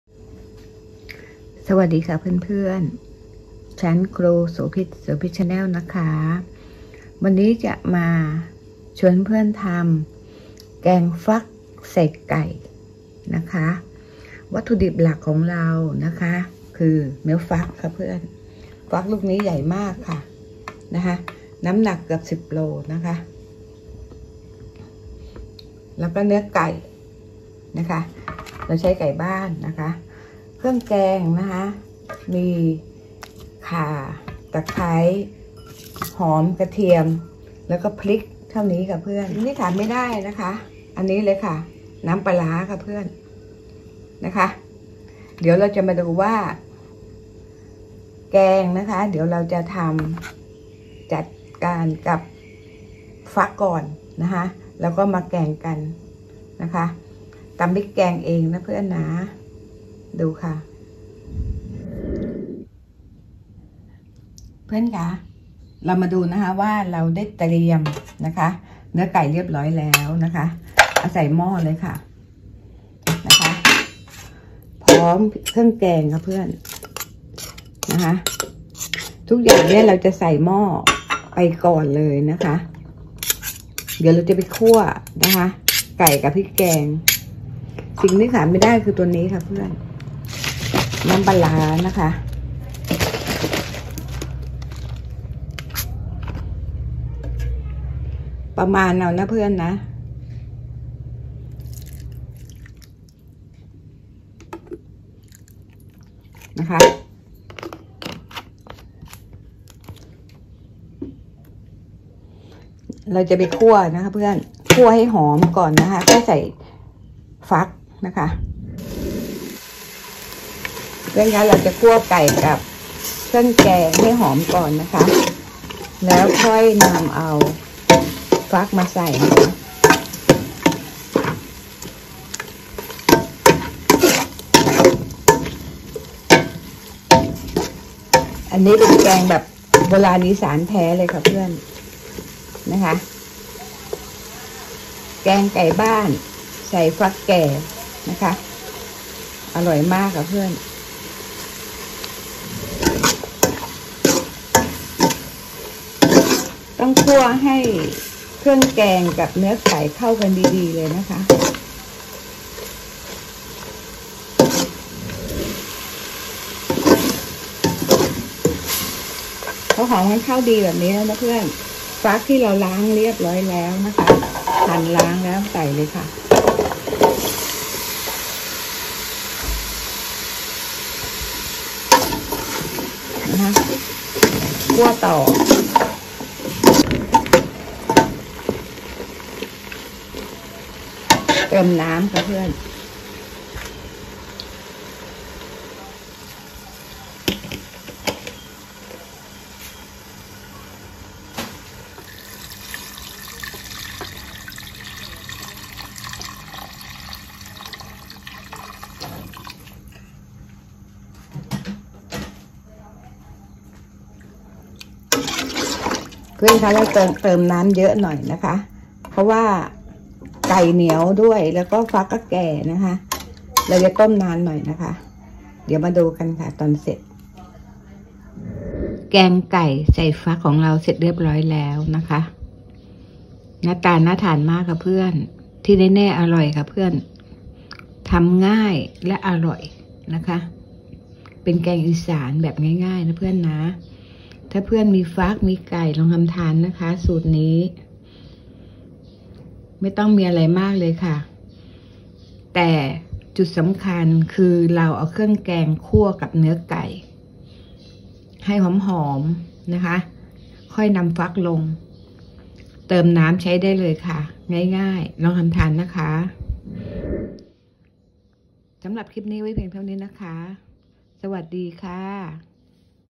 แกงไก่บ้านใส่ฟักเขียว อิสาน
ฟักเป็นผลไม้ จัดอยู่ในกลุ่มผักชนิดต่างๆสามารถนำมาปรุงอาหารได้ทั้ง คาว และหวาน#อาหาร ผลิตภัณฑ์เกษตร อาหารไทย อาหารอีสาน อาหารอร่อย @sopitchannel5693